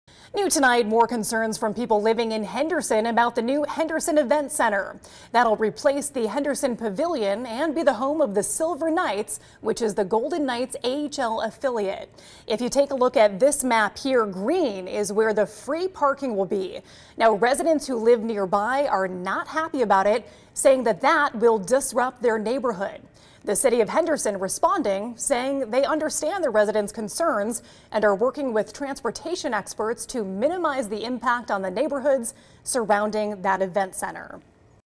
More concerns from Henderson residents over new events center
More concerns from Henderson residents over the new events center that will house the AHL Silver Knights hocket club.